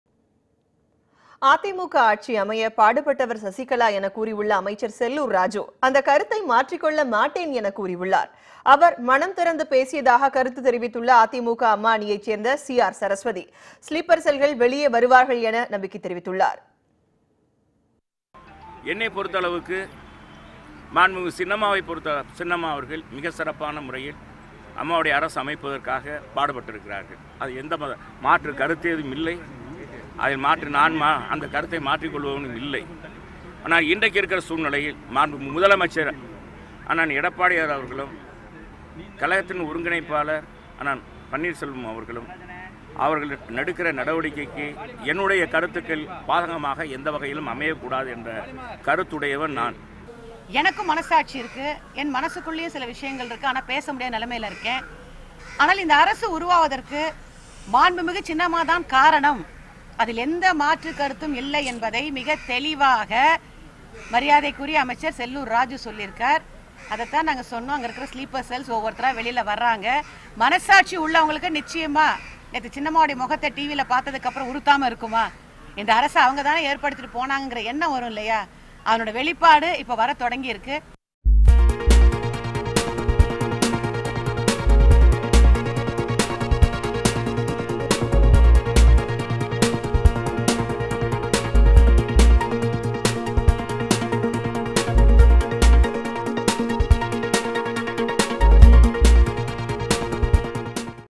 அதிமுக ஆட்சி அமைய பாடுபட்டவர் சசிகலா - அமைச்சர் செல்லூர் ராஜூ
அதிமுக ஆட்சி அமைய பாடுபட்டவர் சசிகலா என கூறியுள்ள அமைச்சர் செல்லூர் ராஜூ, அந்த கருத்தை மாற்றிக் கொள்ள மாட்டேன் என கூறியுள்ளார். அவர் மனம் திறந்து பேசியதாக கருத்து தெரிவித்துள்ள அதிமுக அம்மா அணியைச் சேர்ந்த சி.ஆர்.சரஸ்வதி, ஸ்லீப்பர் செல்கள் வெளியே வருவார்கள் என தெரிவித்துள்ளார்.